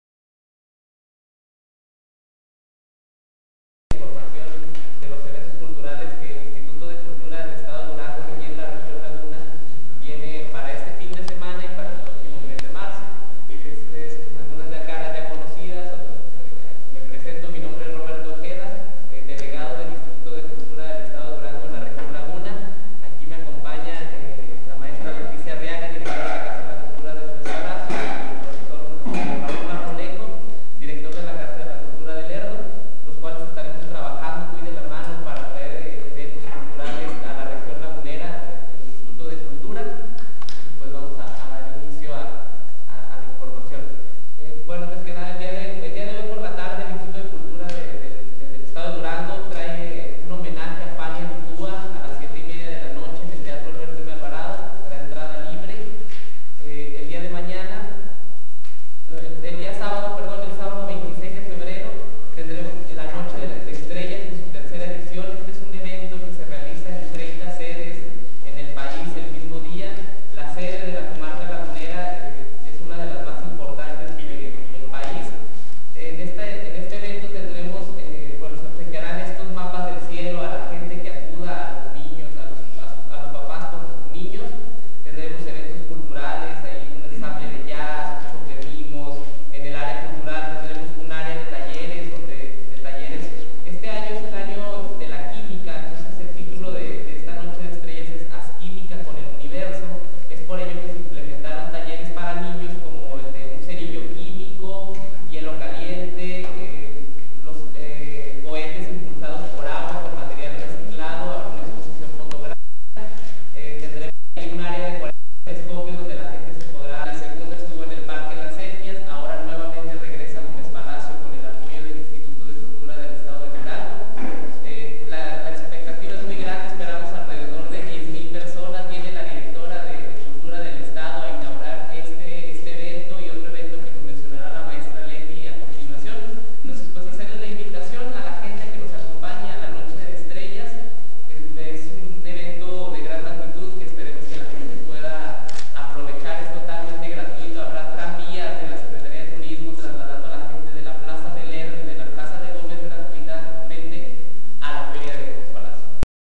Delegado del ICED en la Laguna da a conocer actividades culturales
En rueda de Prensa que se dio en la Casa de la Cultura de Gòmez Palacio, Roberto Ojeda Godoy dio a conocer algunas de las actividades del ICED.